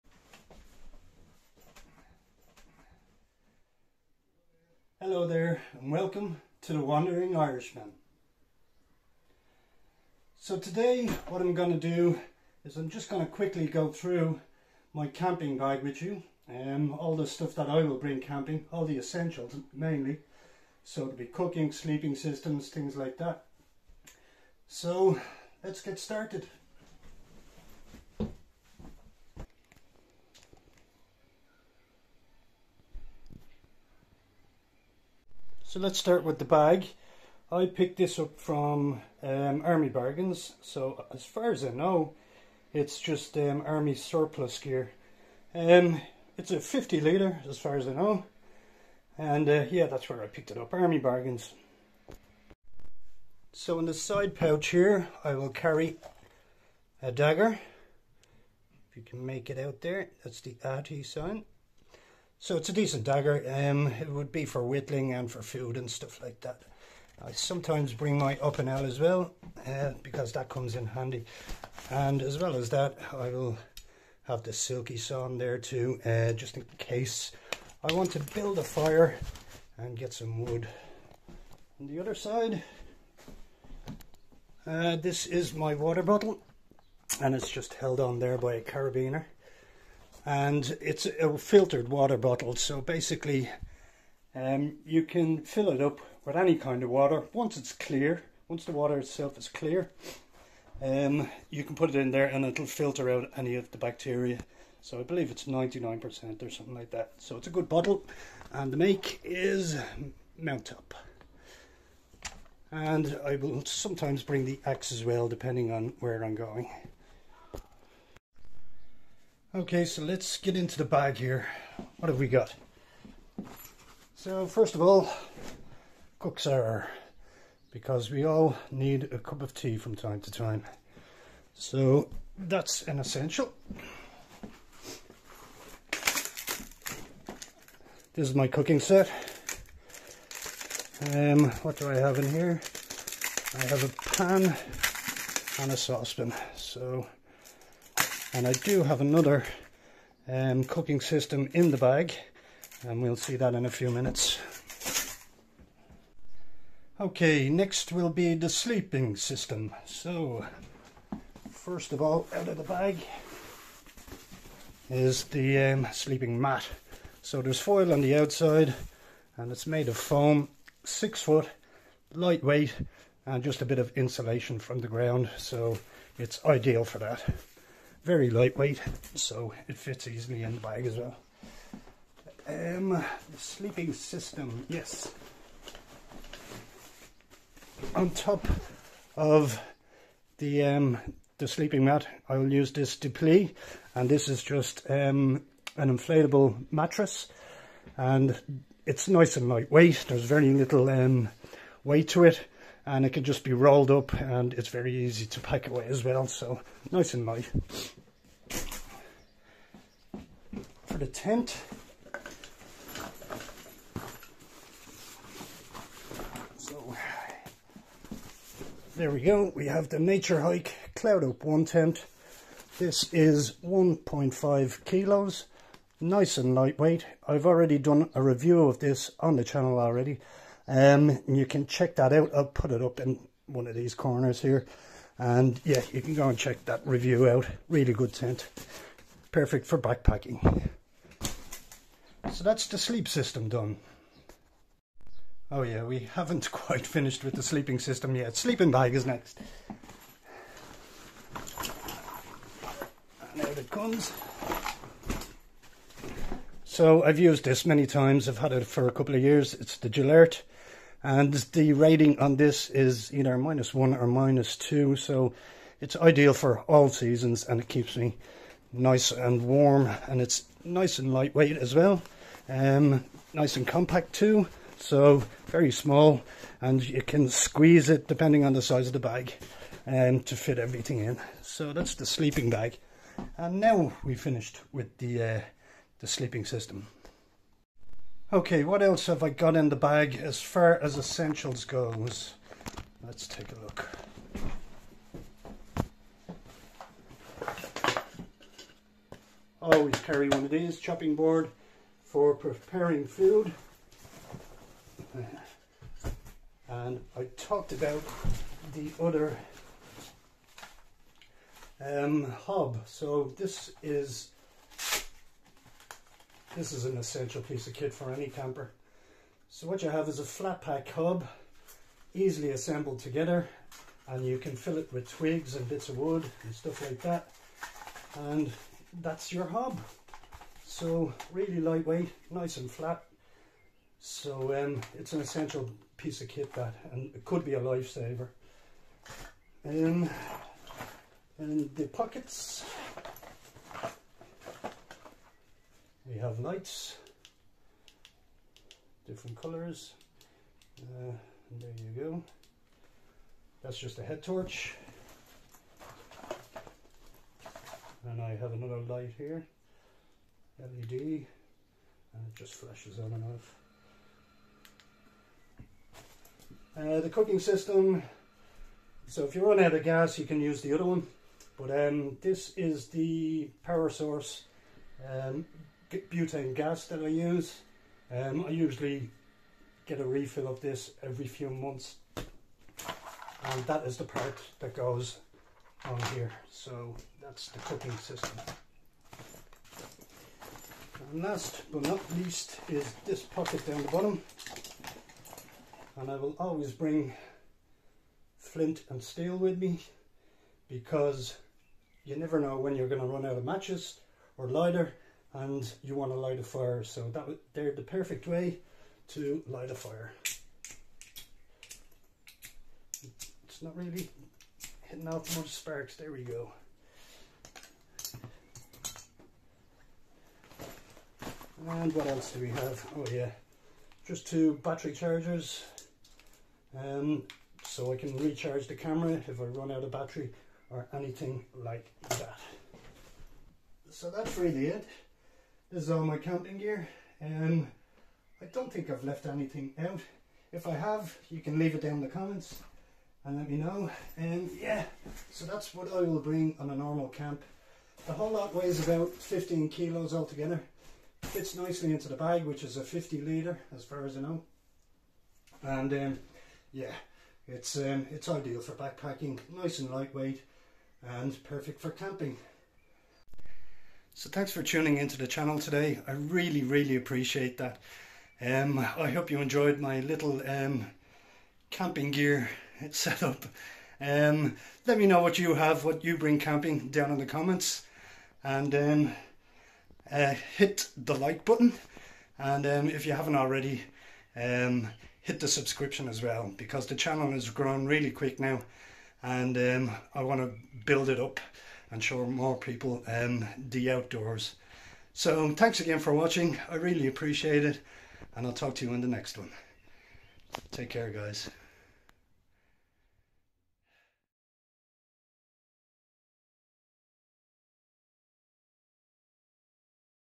Camping Gear Essentials
Just a quick review of my camping gear that I would normally take out while on an over nighter in the outdoors. Many thanks for clicking on the video.

Wood Stove

Inflatable Sleeping Mat

Water bottle
I mentioned in the video that you can use ANY water and it will filter it. Please don't use salt water. Apologies.